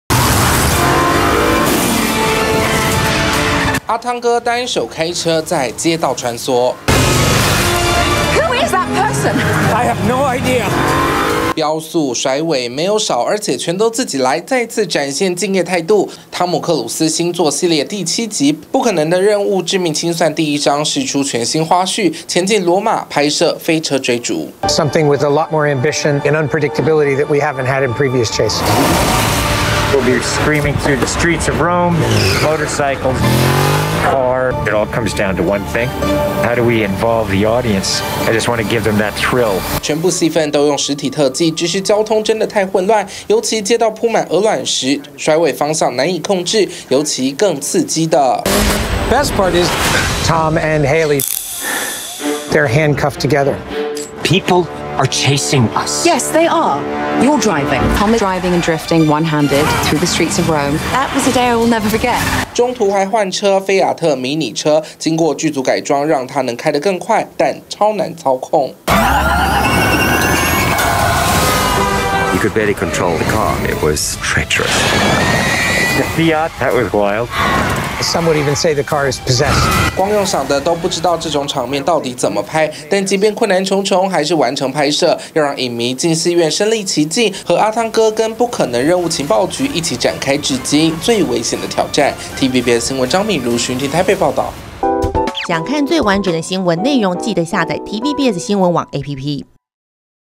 阿湯哥羅馬單手飆車狂甩尾！《不可能的任務7》展飛車追逐｜TVBS娛樂頭條
「阿湯哥」湯姆克魯斯 再度突破自己，新作【不可能的任務：致命清算 第一章】前進羅馬 拍攝飛車追逐，(阿湯哥)單手駕駛，在街頭甩尾飆車，讓觀眾身歷其境